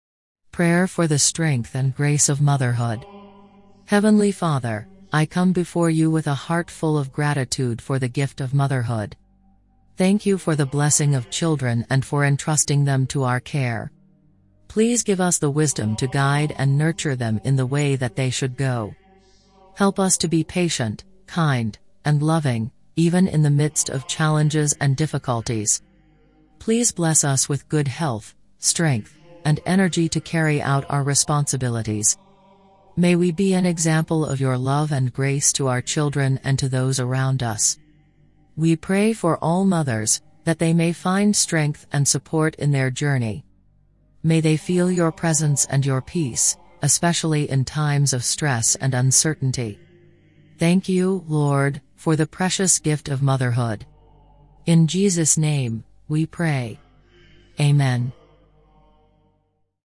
Prayer for the Strength and Grace of Motherhood
Are you a mother looking for strength and grace to navigate the challenges of motherhood? This prayer is for you. Join us in this heartfelt prayer for the strength and grace of motherhood, as we seek the guidance and support of our loving God.

As mothers, we carry many responsibilities and face countless obstacles that can make us feel overwhelmed and weary. But with God's strength and grace, we can face each day with confidence and peace.

In this video, we will pray for patience, wisdom, and compassion as we raise our children. We will ask for the strength to face our challenges and the grace to love our children unconditionally. Join us in this prayer, and may it bring comfort and strength to your heart as you fulfill your role as a mother.